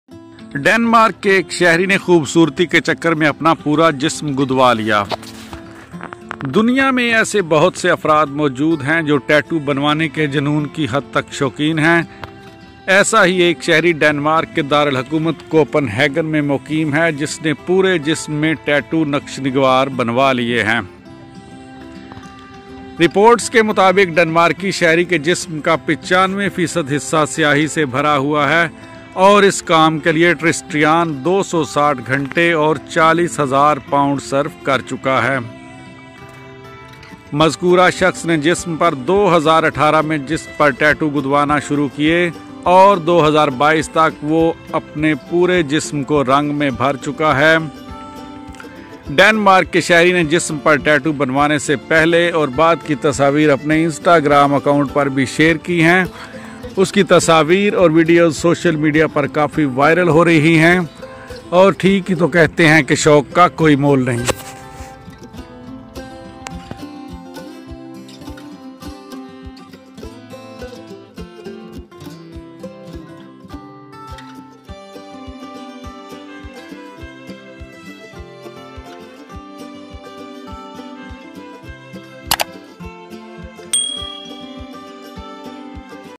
Tristan Weigelt -tattoo addict shares extraordinary transformation after covering 95% of body in ink
US body art addict Tristan Weigelt, 25, has covered 95 per cent of his body in tattoos - only his palms, private parts, the bottom of his feet and ears have not yet been tattooed.
A man who has covered 95 per cent of his body in ink in just four years - enduring an incredible 260 hours under the needle.

US tattoo apprentice Tristan Weigelt started his body transformation in 2018 and it has so far cost the 25-year-old body art addict over £40,000 and left him completely unrecognisable.
He recently shared before and after pictures with his thousands of followers on Instagram, showing his transformation over the years.
Underneath the post one fan commented: “What does your parents say about it? Have you seen them in person yet with the new look?”

Tristan replied: “I have not. My mum would hate it though.”
He added that he had no plans to see his family soon – but he previously admitted to the Daily Star that women find his appearance scary.

“People stare a lot but I don’t notice it as much," he said.
He said: “To anyone out there considering a tattoo, get the tattoo you like.

“Get what you love and what makes sense to you. I see so many people worried about a certain tattoo fitting them or it not being their style. If you like it – then get it...